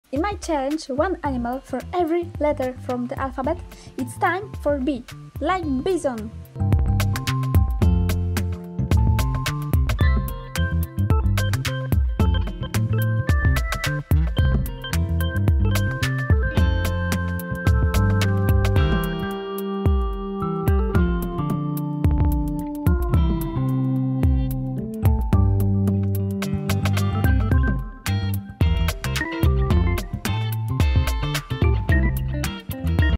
HANDMADE BIDON in clay
I challenged myself to make one animal for every letter from alphabet, this time is for "B" like Bison.